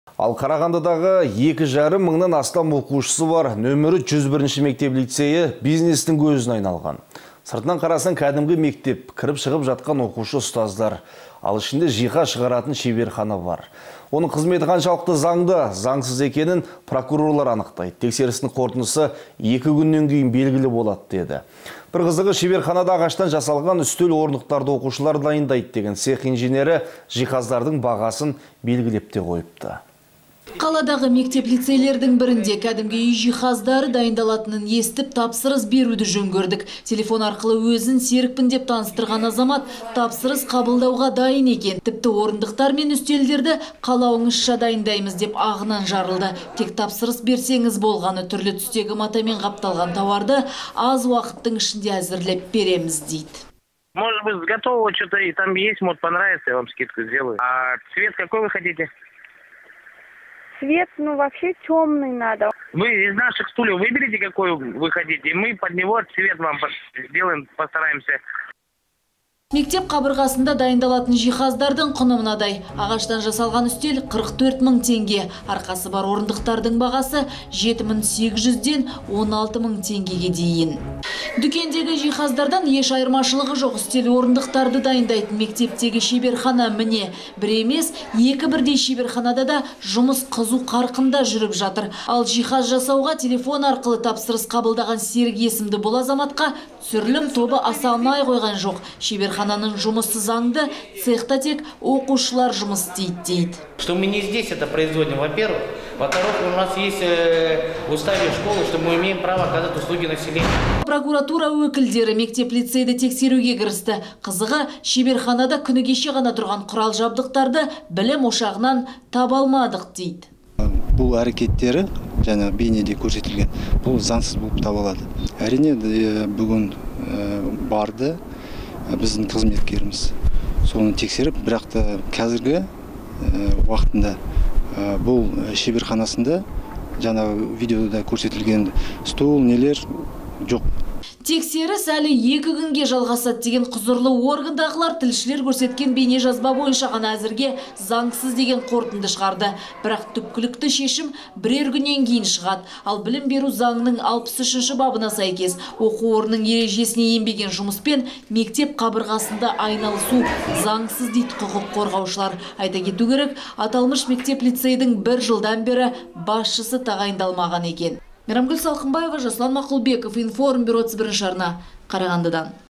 Қарағандыдағы екі жарым мыңнан астам оқушысы бар №101 мектеп-лицейі бизнестің көзіне айналған
Қарағандыдағы екі жарым мыңнан астам оқушысы бар №101 мектеп-лицейі бизнестің көзіне айналған. Сыртынан қарасаң кәдімгі мектеп. Кіріп-шығып жатқан оқушы ұстаздар. Ал, ішінде жиһаз шығаратын шеберхана бар екен.Оның қызметі қаншалықты заңды, заңсыз екенін прокурорлар анықтайды. Тексерістің қорытындысы екі күннен кейін белгілі болады деді.   Бір қызығы, шеберханада ағаштан жасалатын үстел-орындықтарды оқушылар дайындайды деген цех инженері жиһаздардың бағасын белгілеп қойыпты.